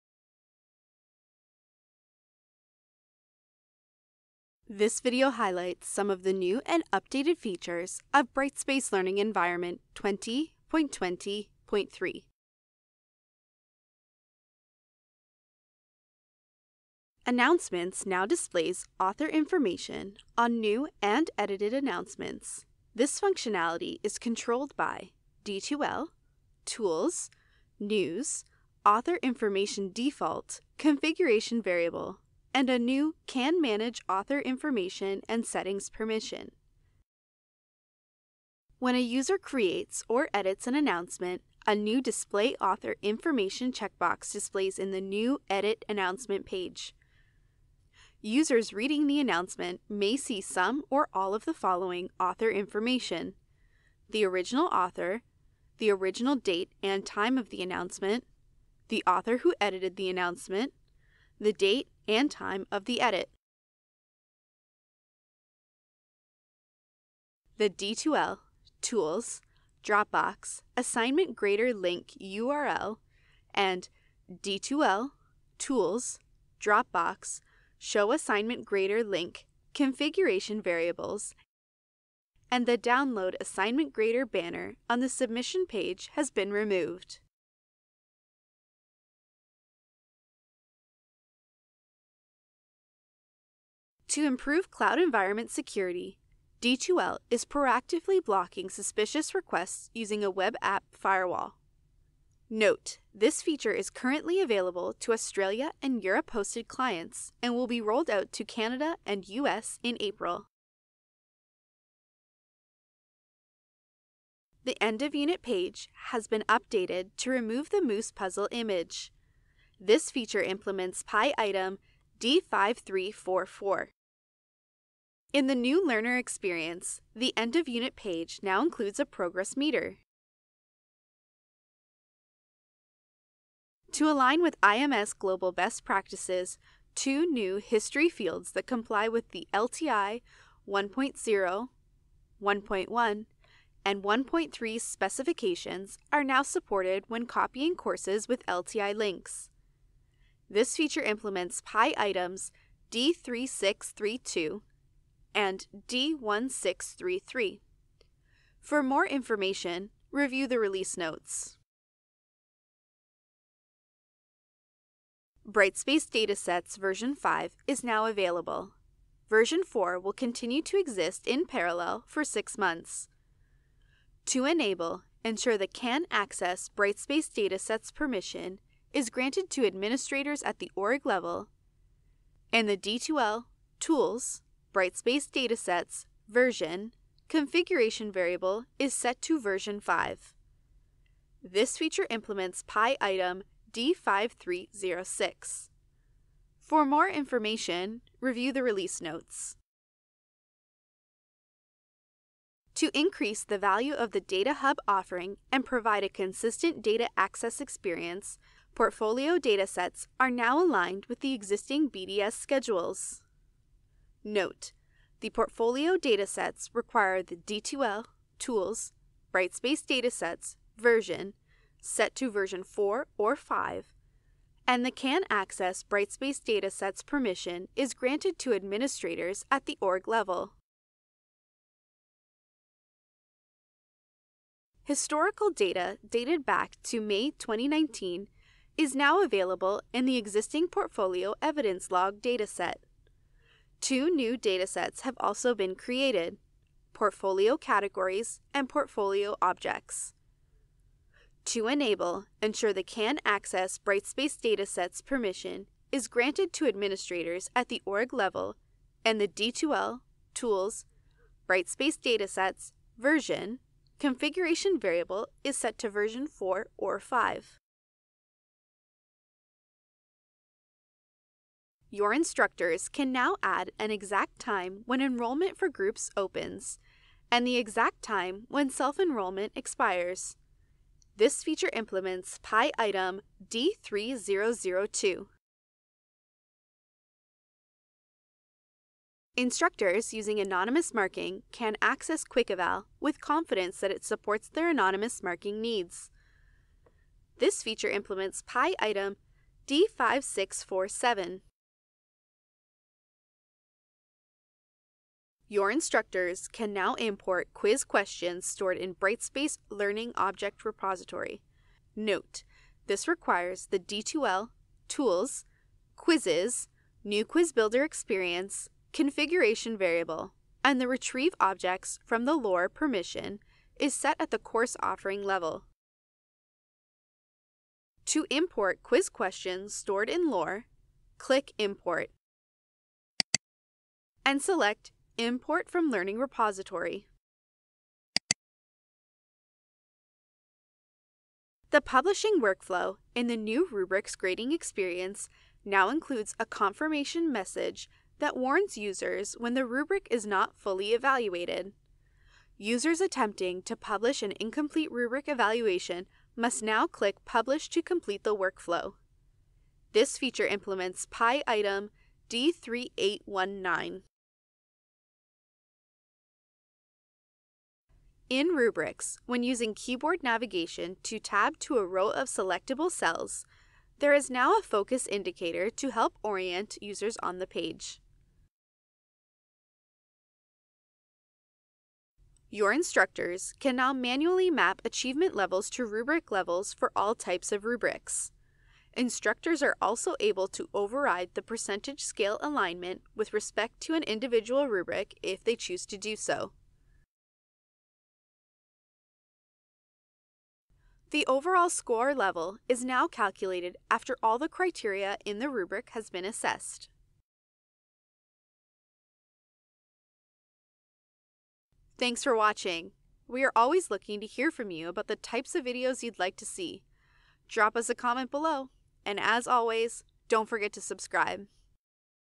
Release Highlights - March 2020 (v.20.20.3) - Administrator
This video highlights some of the new and updated features of Brightspace Learning Environment 20.20.3.

Table of Contents:
Announcements - Display author information | New 0:17
Assignments - Download Assignment Grader banner | Removed 1:06
Brightspace Learning Environment - Improvements to Security using a WAF | Updated 1:33
Content - End of Unit pages | Updated 1:54
Copy Course Components - LTI links preserved in copied courses | New 2:12
Data Hub - Brightspace Data Sets Version 5.0 | New 2:44
Data Hub - Portfolio Evidence Log Brightspace Data Sets | Updated 3:21
Data Hub - Portfolio Evidence Log, Portfolio Categories, and Portfolio Evidence Objects Brightspace Data Sets | New 3:53
Groups - Set exact time learners can self-enroll | Updated 4:30
Quick Eval - Support for anonymous marking | Updated 4:48
Quiz Builder - Import questions from Brightspace Learning Repository | New 5:03
Rubrics - Confirmation message for partially unevaluated rubrics | New 5:40
Rubrics - Improved accessibility in Rubric grading | Updated 6:07
Rubrics - Manually map achievement levels to Rubric levels | Updated 6:19
Rubrics - Overall Score levels calculated after rubric criteria completion | New 6:39